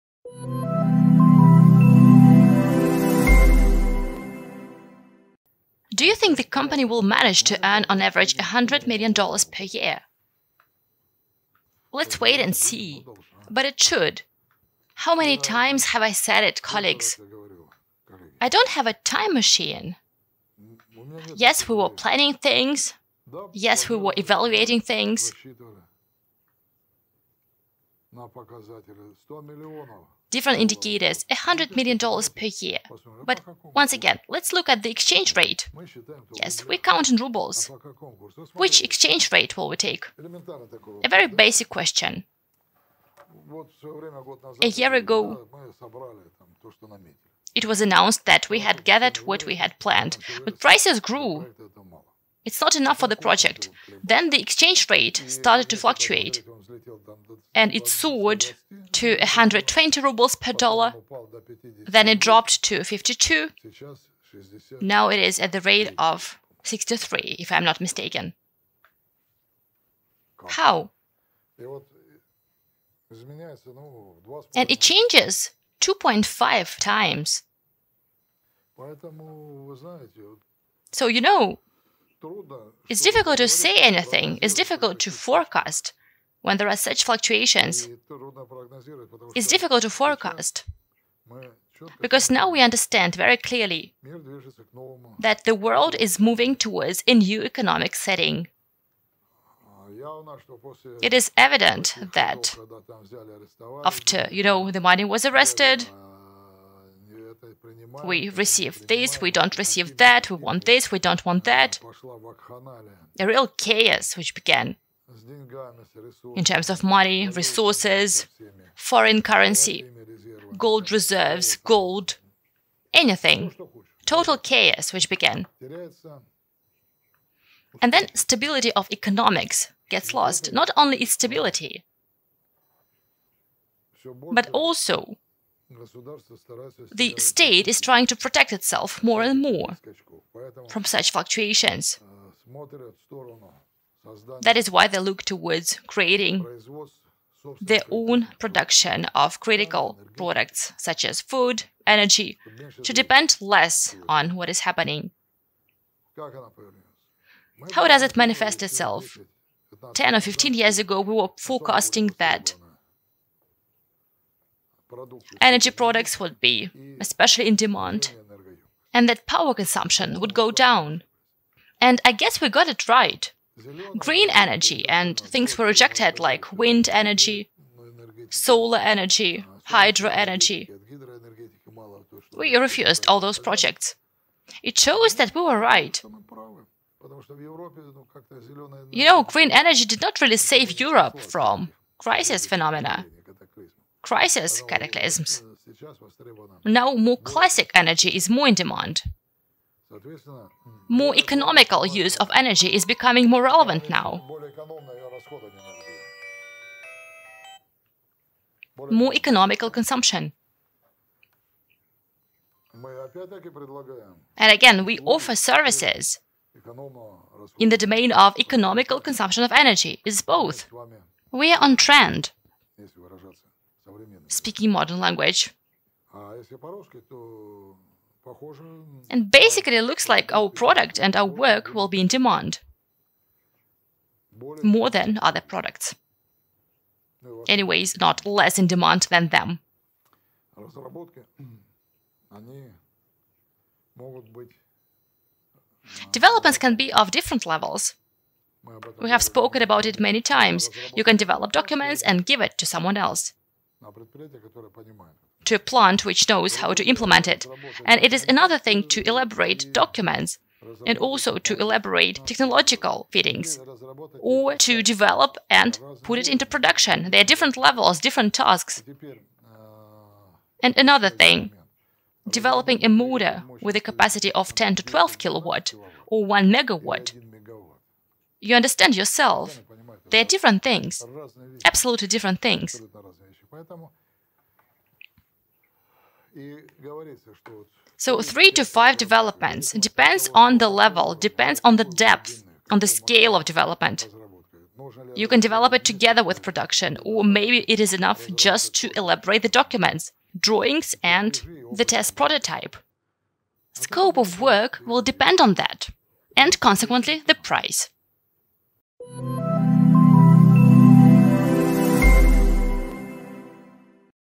Q&A / How much can the "Sovelmash" D&E earn?
Many of the project investors are interested in what income the "Sovelmash" design and engineering technology department will generate by developing custom-made electric motors.

It depends on several factors

• Development complexity. It is one thing to develop a motor and give its documentation to the customer company. Another thing is to develop the tooling along with the documentation or help the customer set up the motor production. These are tasks with different levels of complexity and different costs. The cost of development is determined, among other things, by the motor dimensions. Depending on the complexity and scale of development, the "Sovelmash" D&E will be able to execute up to 3-5 orders per year. The cost of developing a motor averages $50,000,000 worldwide.

• External circumstances. The economic situation in the world affects the demand for "Sovelmash" products and services. It is difficult to make accurate predictions in times of change, but the general trends are obvious. States seek self-sufficiency and efficiency in vital industries, including energy production. And "Sovelmash" can help them with that. For example, to build an enterprise to produce energy-efficient motors that are superior to all existing analogues in the world. The demand for "Sovelmash" services is growing.

Conclusion: It is impossible to give an accurate prediction of how much the "Sovelmash" D&E will earn. But an average income of a few dozens or hundreds of millions of dollars a year is quite feasible.

Watch a video in which the project leader and creator of the "Slavyanka" technology Dmitriy Duyunov answers in more detail the question about the income level of "Sovelmash".